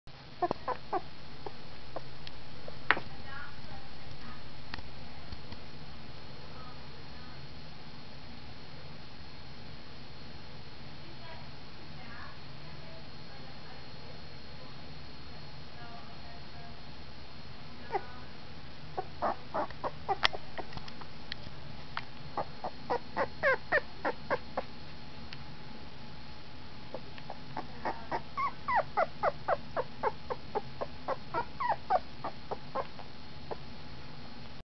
J puppy 15 days old
15 day old red nose brindle pit bull puppy